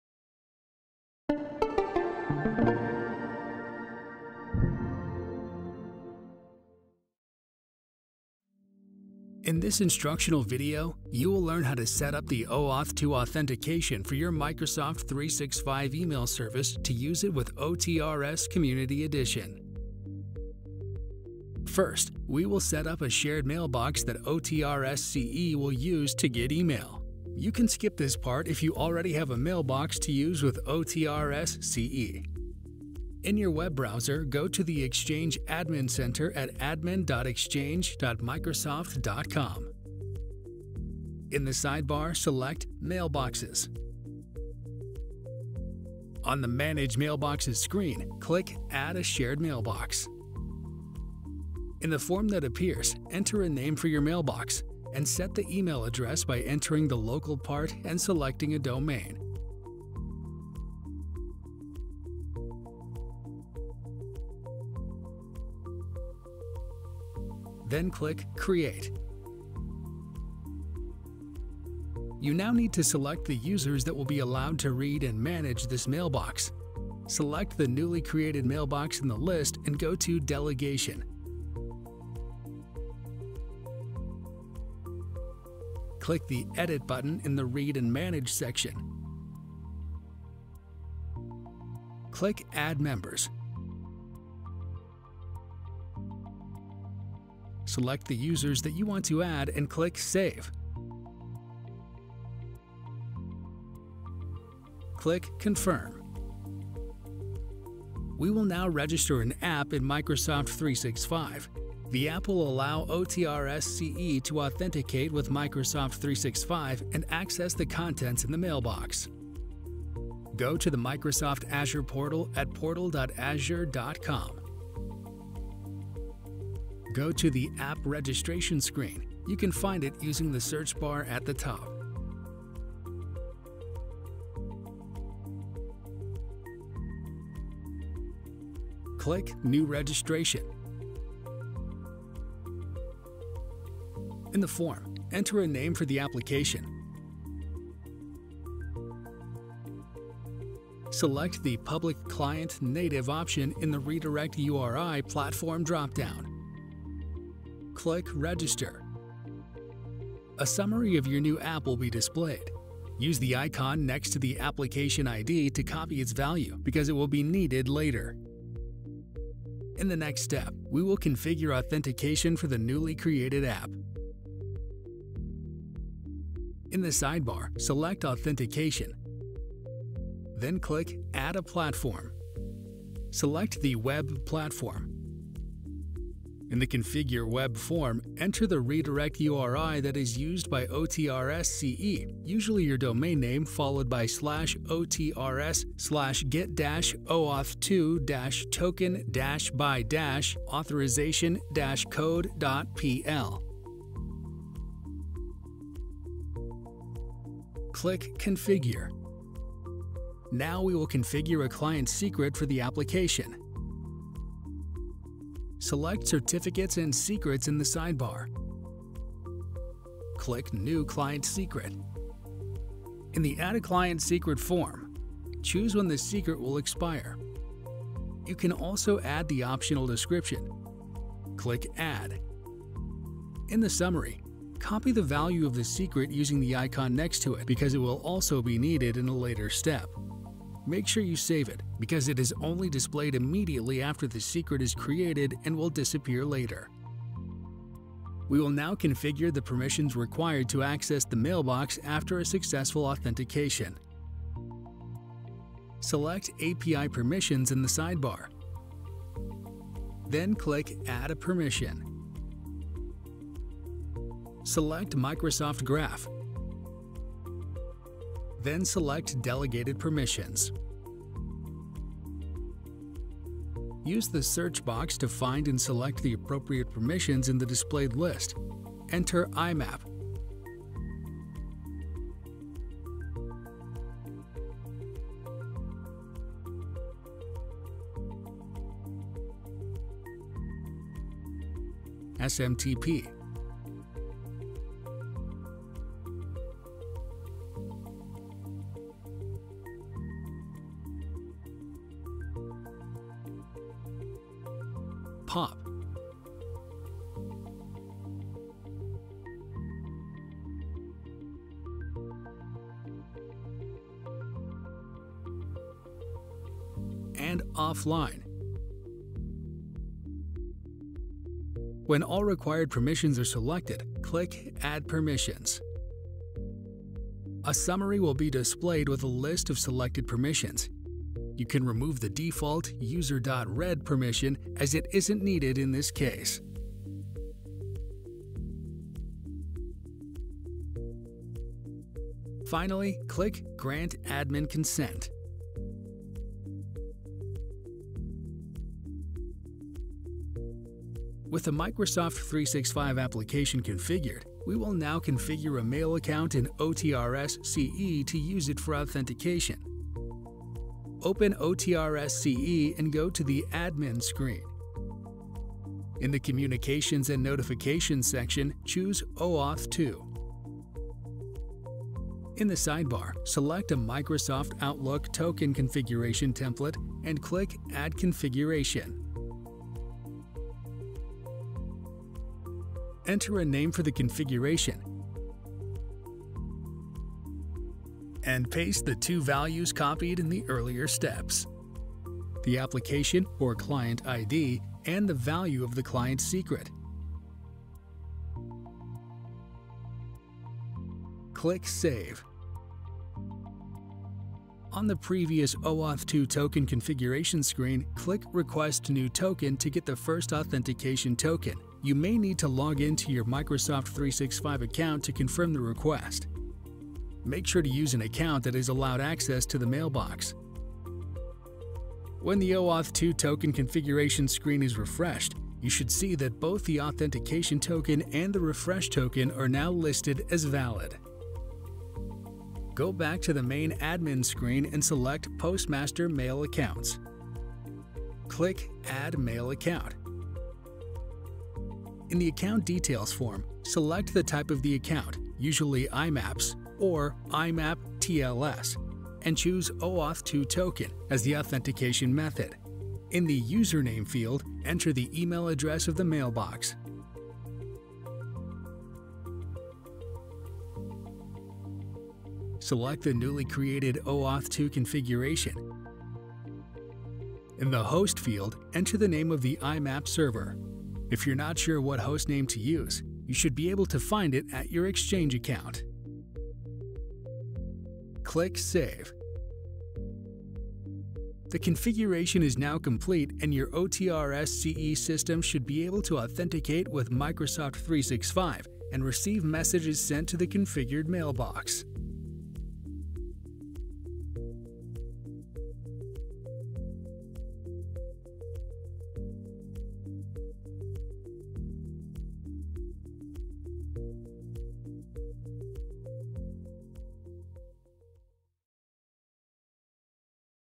((OTRS)) Community Edition - Setting up OAuth2 authentication for Microsoft 365 mailbox
This video shows how to configure OAuth2 authentication for Microsoft 365 (formerly Office 365) email accounts in ((OTRS)) Community Edition.